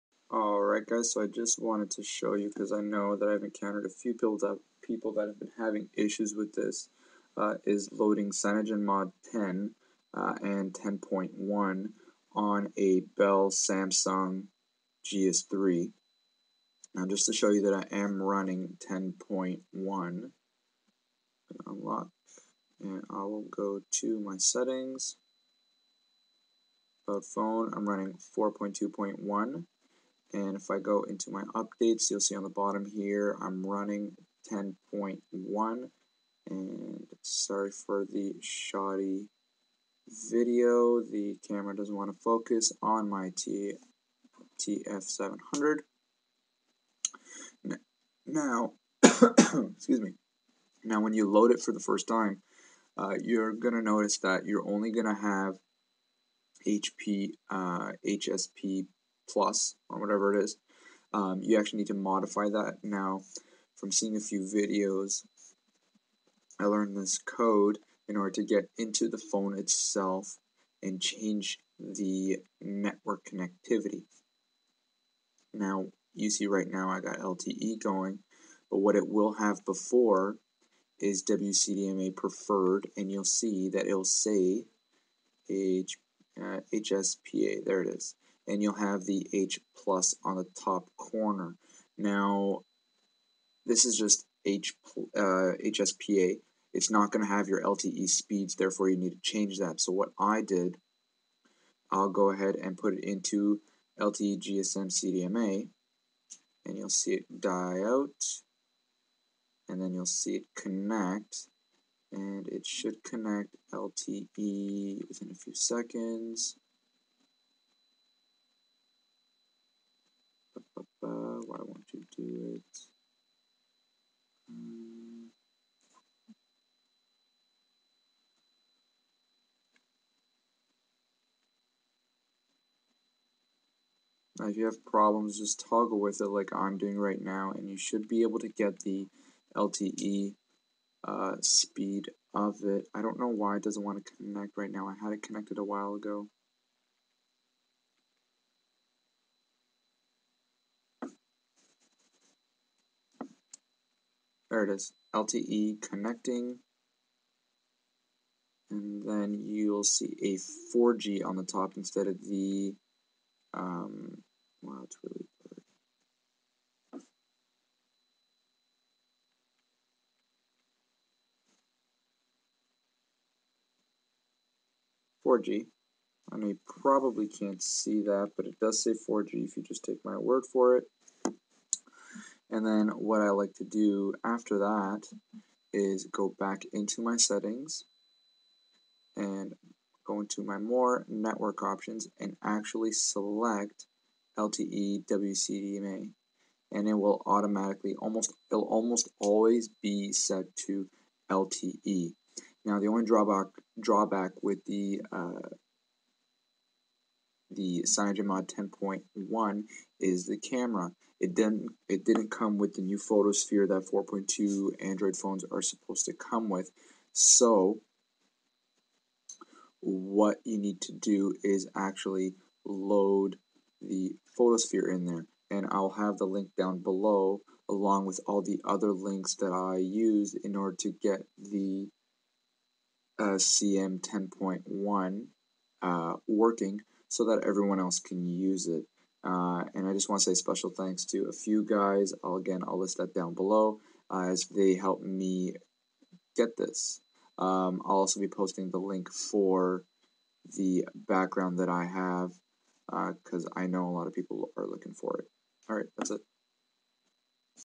Samsung GS3 Bell - CM 10.1
Showing my Bell Samsung GS3, rooted with CM 10.1 installed.

I can only confirm that this will work with Bell Samsung GS3.

Steps:
1) Get the latest version of Odin (See link below)
2) Get AT&T (SGH-I747) - AT&TGS3CWM6.0.1.2.tar (See link below)
3) Get latest Team Epic's Root from Recovery (See link below)

If you want to get new gallery and camera app;
4) Get Photo-sphere zip file.

Install How To:
1) Put Team Epic's Root from Recovery in Downloads folder (or somewhere you can easily find it)
2) Unplug Phone.
3) Open ODIN.
4) Turn phone off.
5) Put into download mode by holding down volume button, home button and power button. Once you see the first screen let go.
6) Read the screen, make sure you agree that you are voiding your warranty.
7) Press up volume button to agree and proceed. You are going to see a picture of an android.
8) Plug phone into computer with ODIN already running. It might start installing drivers, let it finish.
9) Uncheck "Auto-reboot".
10) Select PDA and it will ask you to select a file. Choose the AT&T (SGH-I747) - AT&TGS3CWM6.0.1.2.tar.
11) Hit Start. It shouldn't take too long.
12) Once it says PASS, unplug phone from computer and pull the battery to turn off phone.
13) Turn phone on into recovery mode by holding volume up button, home button and power button until you see the first screen, then release all buttons. Wait for it to load into CWM.
14) Now go to Install from SD (or something like that). (Use the volume up and volume down to navigate and the power button to select options).
15) Choose zip file and select the Team Epic's Root from Recovery. It will then ask you to select Install.
16) Once installed, you can reboot your phone.

Your phone is now rooted. You can now flash to other things like CM 10.1/CM 10.

When flashing others, if you get an aborted error it is because of the type of CMW you are using. The one I have listed below is the one that worked for me when flashing CM 10.1 (AT&T Variant).

Special Thanks to:
xdadevelopers
qbking77
HaligonianType1
Marques Brownlee